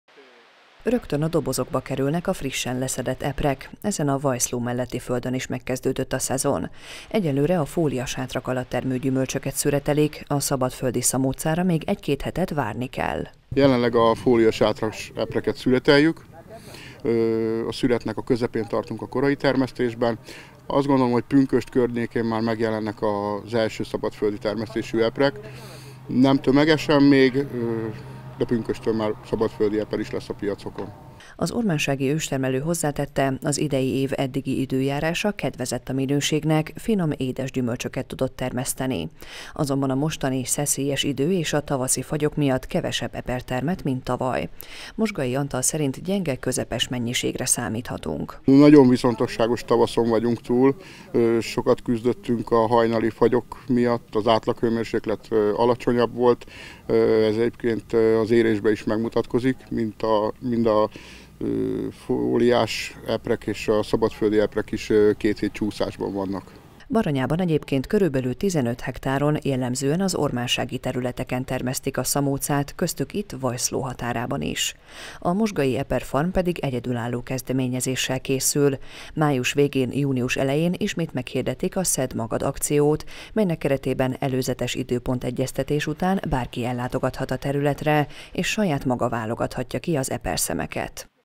Baranyai eper - Híradó - 2021.05.17.
Gyenge-közepes mennyiségű eperre számíthatunk – tudtuk meg Mozsgai Antaltól. Az ormánsági őstermelő földjén még a fóliasátras szamócákat szüretelik, de pünkösdre már a szabadföldi gyümölcs is kapható lesz a piacokon. A minőség az itthon megszokott, így Baranyában is finom, édes epret vásárolhatunk.